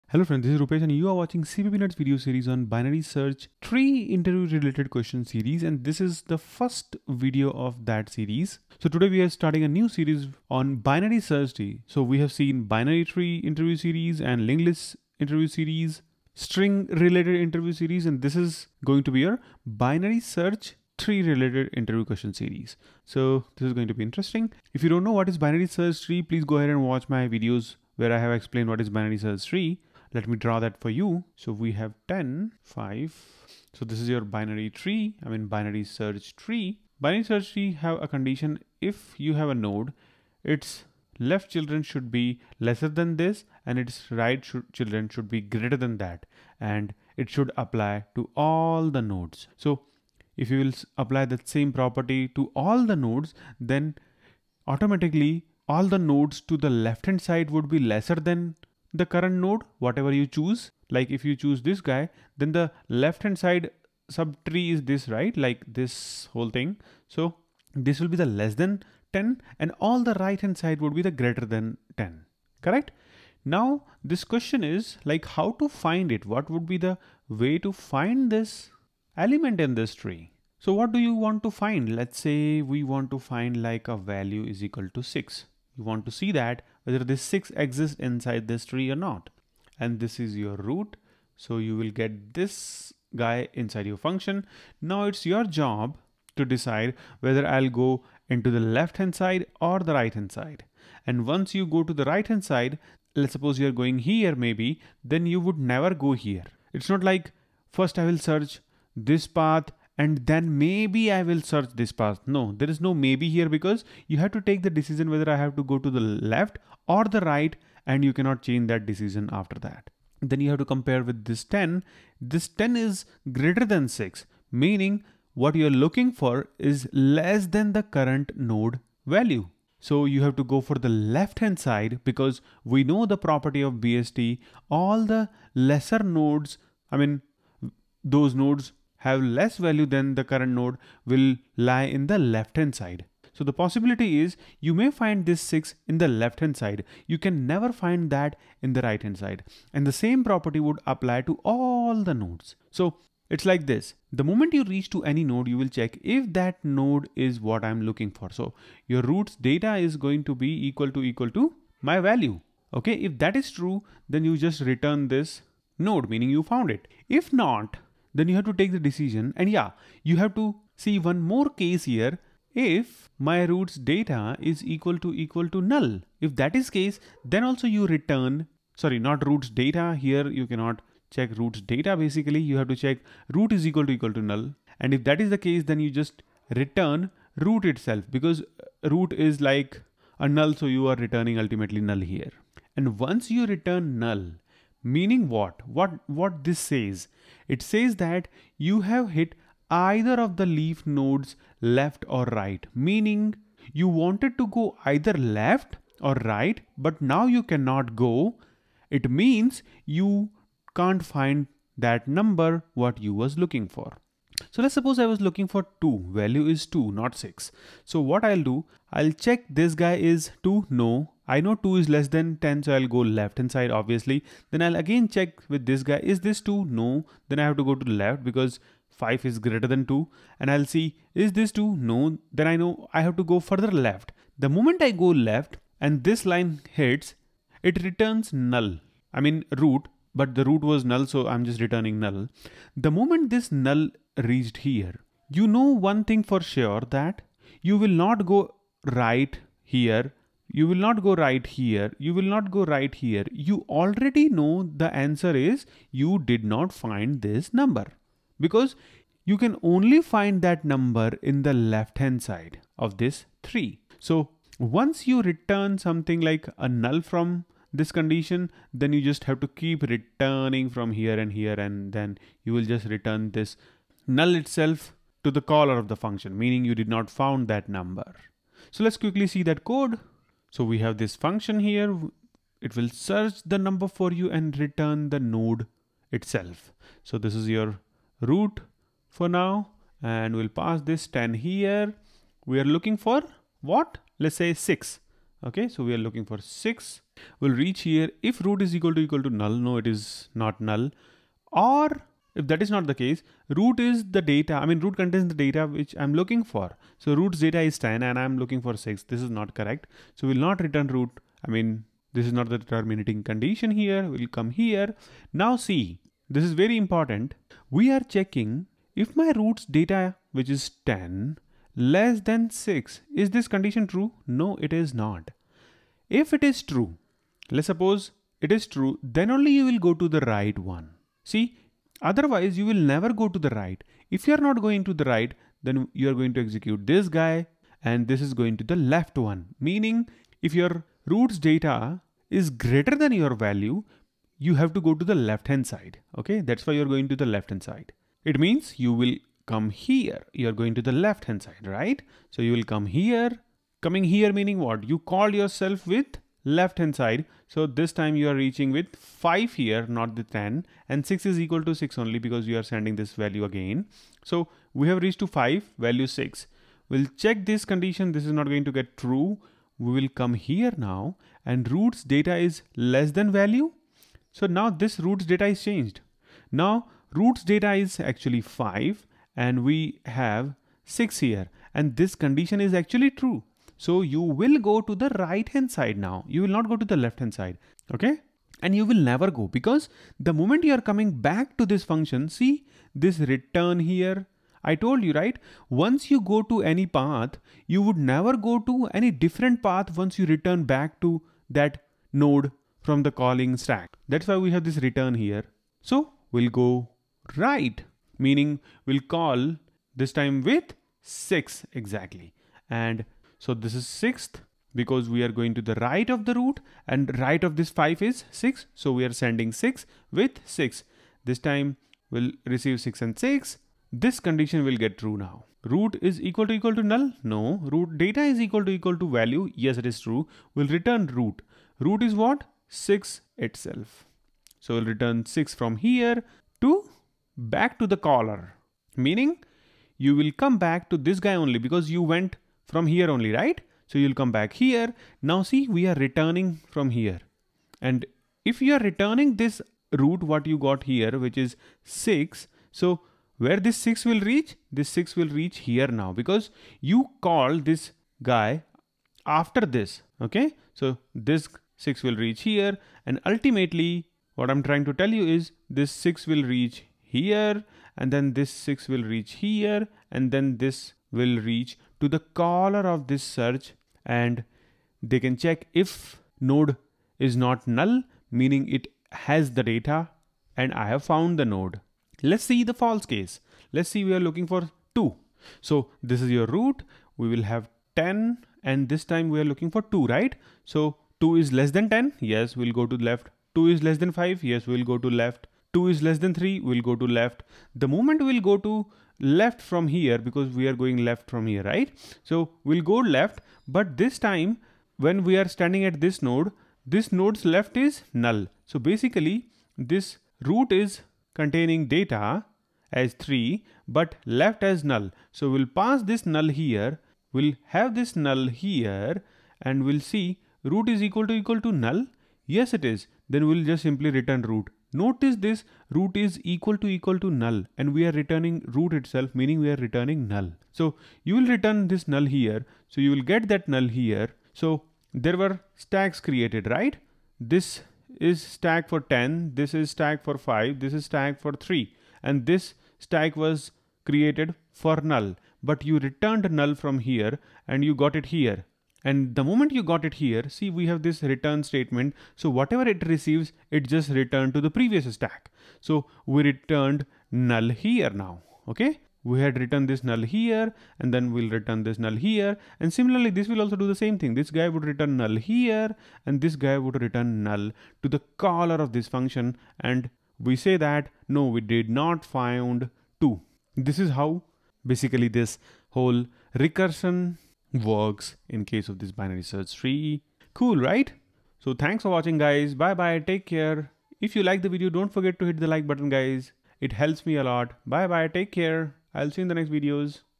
Search Element In Binary Search Tree (BST)
BST is a fantastic tree data structure, it stores data in sorted form.
Nodes value to the left of the current node are lesser and nodes value to the right side of the current nodes are greater. On this basis we construct the whole tree.
And in this video we will learn how to find any element exist in the given binary search tree (BST), this video uses recursion to find element in BST.